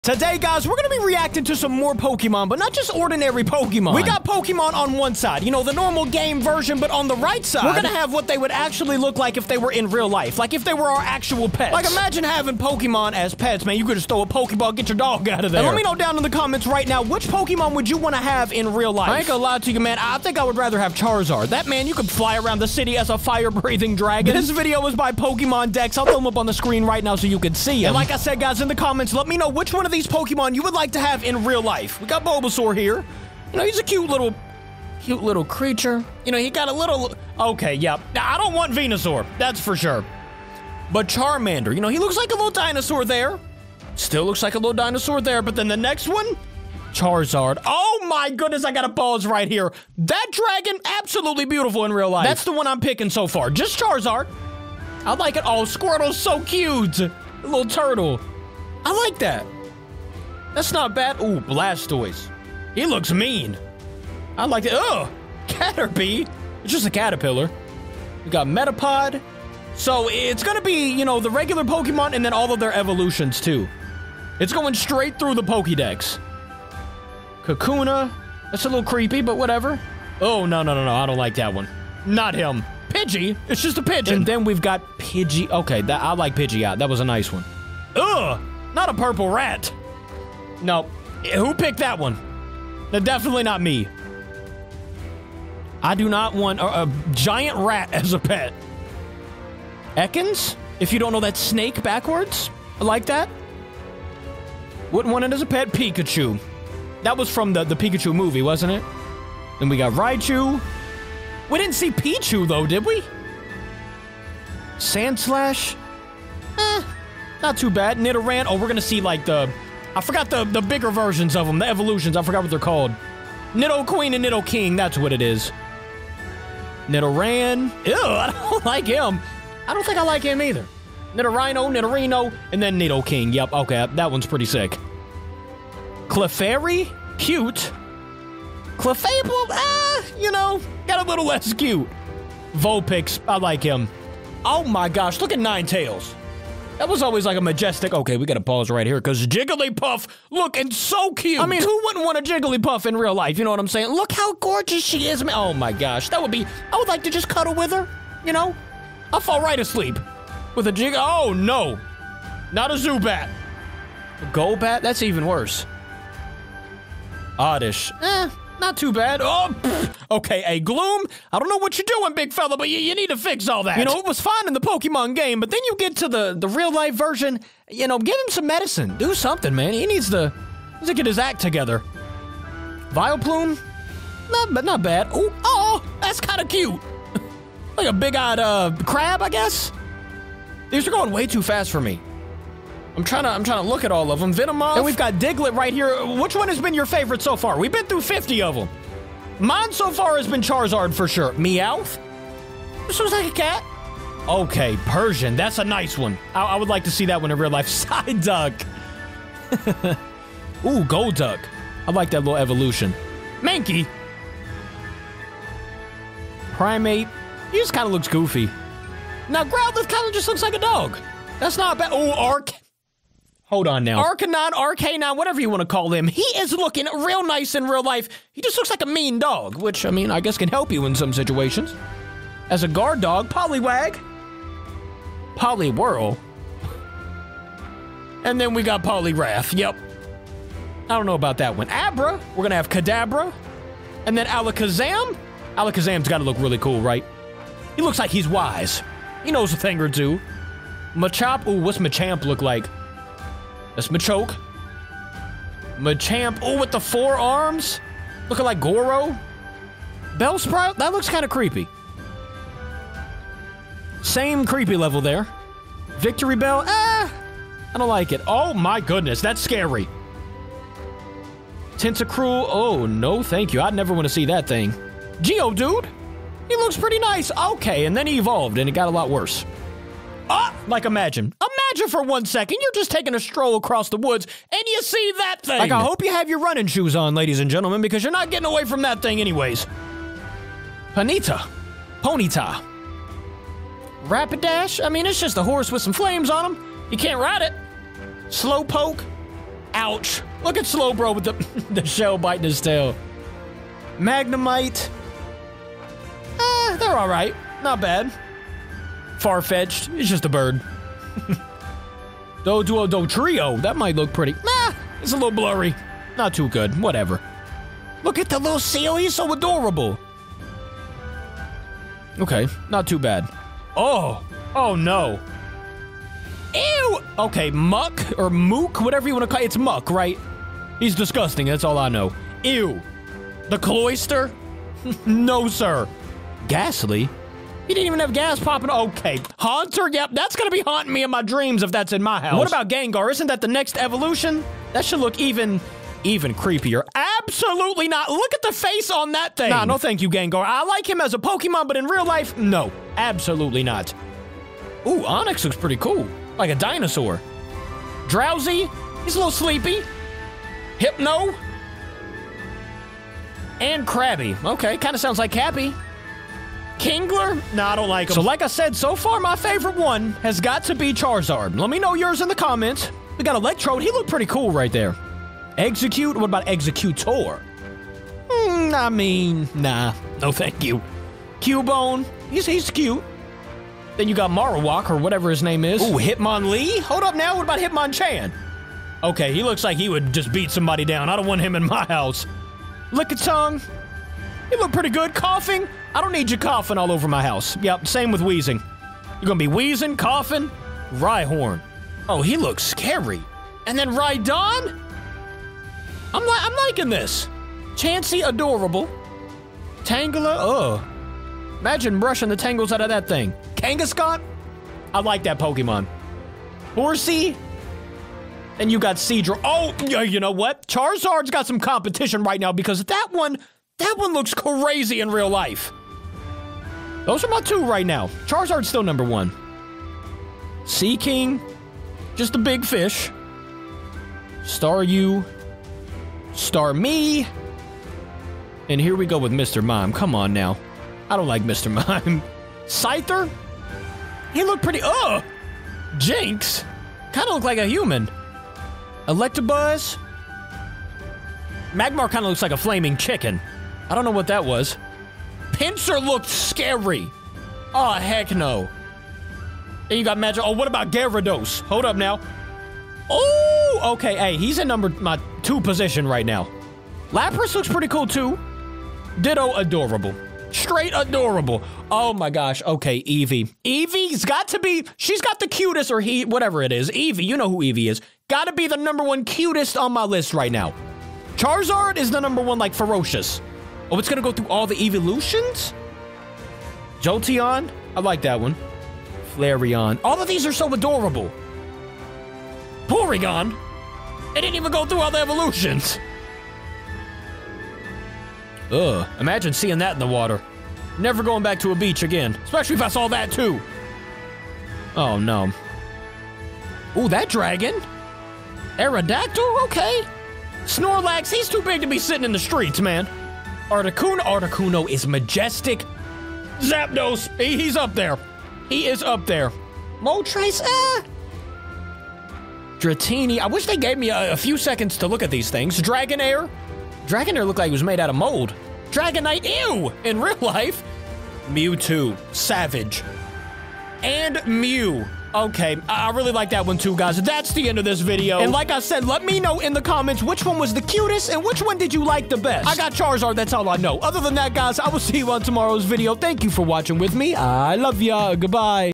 Reacting to POKEMON but in REAL LIFE!
Reacting to what POKEMON would look like in REAL LIFE!! Which one if your FAVORITE??